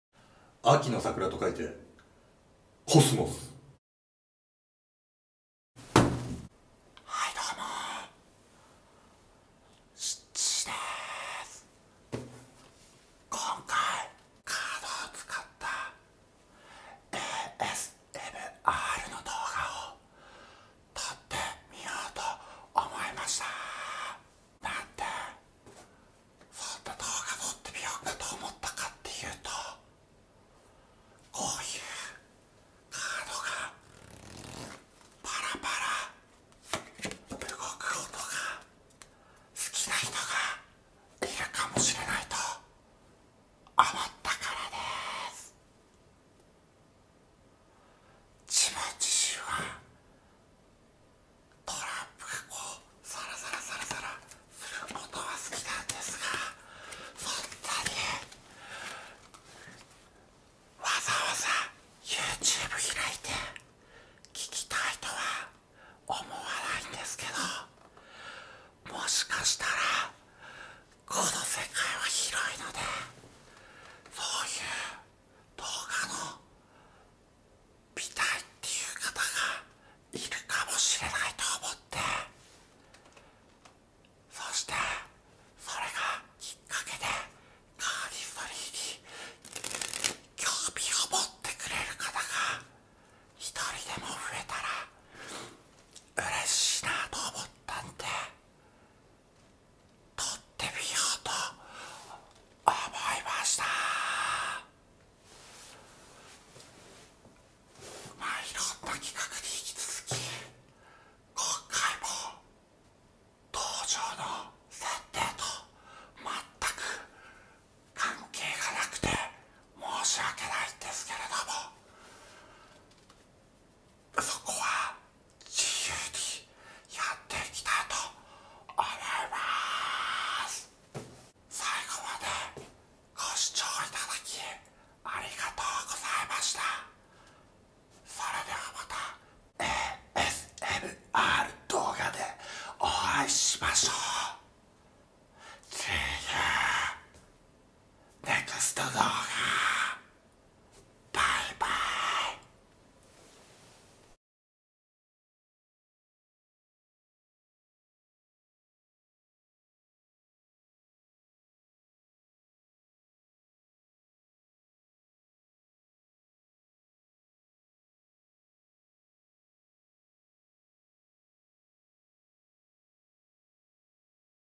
【Cardistry x ASMR】新コーナー始めます
色々なデックを使ってやる予定なので
微妙な音の違いをお楽しみいただけると嬉しいです。